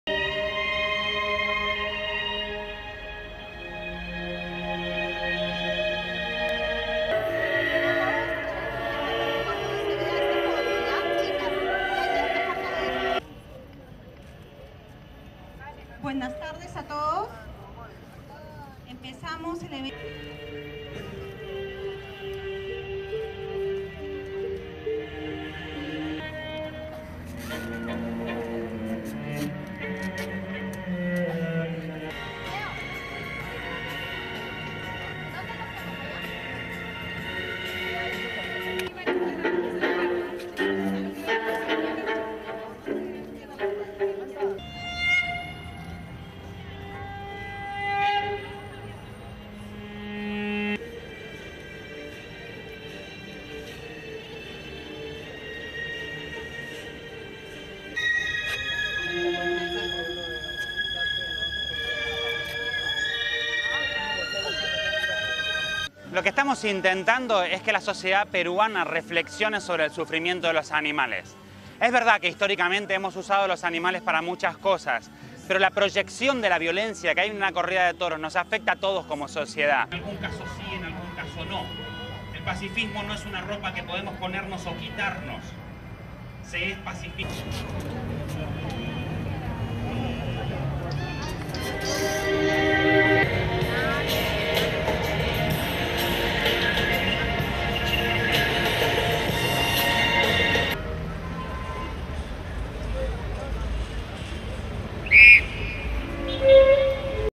Protests against bullfighting take place in central Lima
1. Wide of animal rights demonstration in central plaza
2. Mid of animal rights protesters lying on ground partially dressed, with fake blood and banderillas (barbed short spear used on bulls)
3. Mid of protesters with banner in background reading (Spanish): "Enough Already"
4. Mid of people looking at protest
5. Close of sign reading (Spanish): "Enough Already" held by protester
6. Close of fake bandilleras taped onto protesters
7. Close of fake blood and taped-on bandilleras on a protester's back, with bandilleras falling over
8. Close of sign in English and Spanish reading: "Bullfight Abolition"
9. Mid of people looking at demonstration
10. Close of woman in protest with fake banderillas and blood on her back
11. SOUNDBITE (Spanish) Leonardo Anselmi, Anti-bullfighting Activist:
"What we hope is that Peruvian society reflects on the suffering of the animals. It is true that historically we have used animals for many things, but the the violence of a bullfight affects all of society."
12. Wide of protest with Anselmi speaking
13. Close banner with words in Spanish, Italian, French and English reading: "Torture isn't art or culture"
14. Wide of protest and reporters
15. Wide of protest with crowd gathered around
STORYLINE:
About 100 demonstrators gathered in a central plaza in Lima, Peru on Saturday to call for an end to bullfighting.
The animal rights activists wore only black shorts and lay in a jumbled mass on the ground.
Fake blood was spattered across their bodies and fake bandilleras, the short barbed spears used to weaken and anger bulls in bullfights, were taped to their backs.
Around the protesters there were signs reading: "Bullfight Abolition" and "Torture isn't art or culture".
One female protester held a sign reading "Enough Already" over her body.
Bullfighting has a long history in Peru.
The tradition was brought to the South American country by the Spanish and has deep roots among upper class Peruvians.
Critics say thousands of bulls suffer painful deaths each year in bullfighting.
"What we hope is that Peruvian society reflects on the suffering of the animals. It is true that historically we have used animals for many things but the the violence of a bullfight affects all of society," said Leonardo Anselmi, an anti-bullfighting activist.
Supporters say that it is a part of Peruvian culture and and an art form.